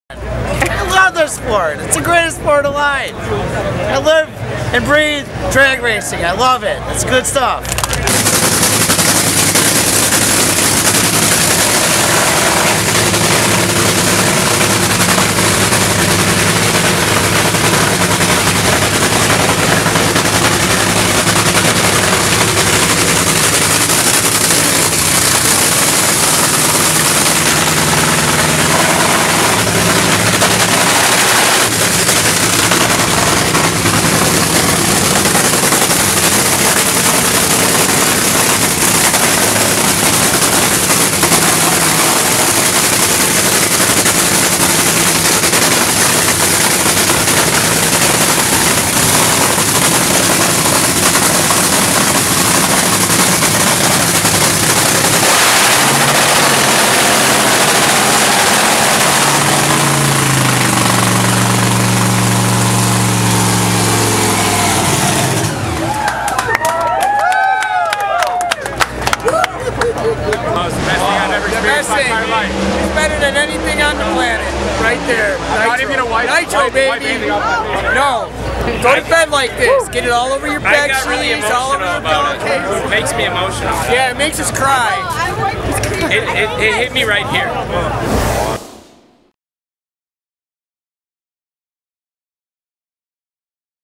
Smell the Nitro! Chicago Fans have Love for Drag Racing
There's nothing like the smell of nitro and these fans tell it like it is.  If you've never been to a Mello Yello Drag Racing series event, well these fans will tell you why they love it and watch Bob Bode warm-up at the 2014 O'Reilly Auto Parts Route 66 NHRA Nationals.